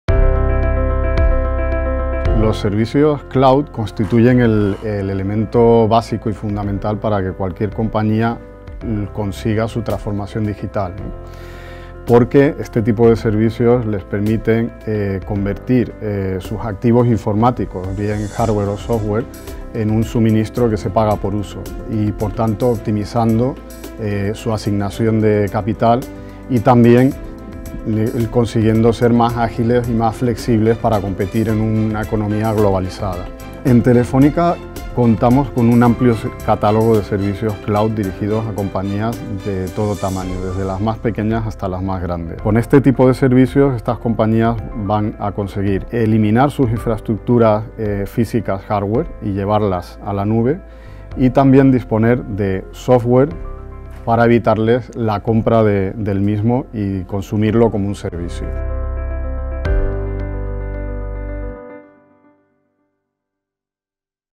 ¿Cómo ayudan los servicios Cloud a la transformación digital?
José María Cuellar,  director Global de Cloud B2B, comparte con nosotros la visión que tiene Telefónica de la transformación de las empresas en el mundo digital.